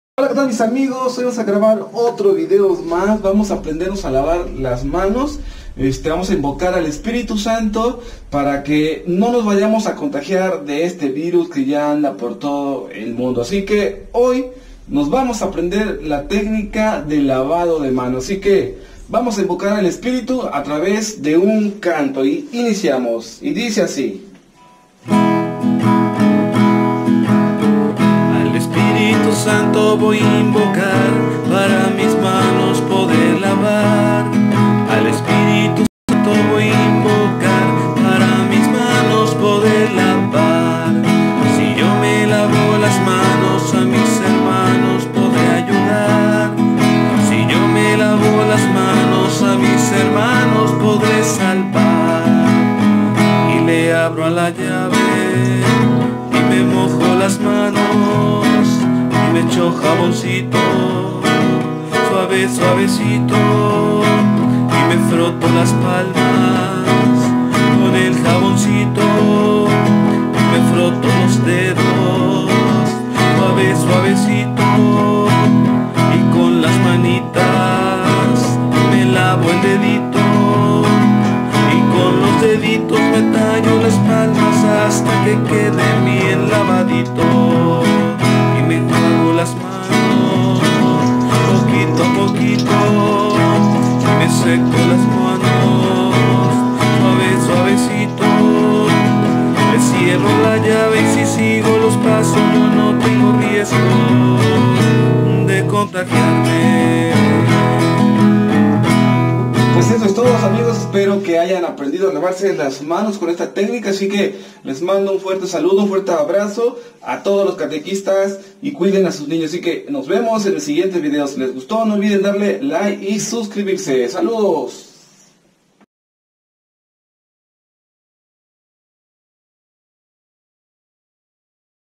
Aprendamos a lavar las manos invocando al Espiritu Santo
Es un video para que los niños se aprendan a lavar las manos a traves del canto, seamos responsables y sigamos las indicaciones para que no se propague mucho el virus.